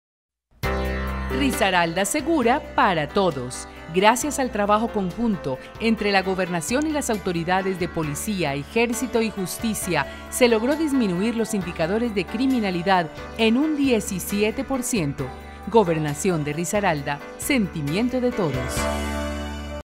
Risaralda Segura para Todos baja criminalidad
Autoridades en Risaralda lograron bajar los índices de criminalidad en el departamento.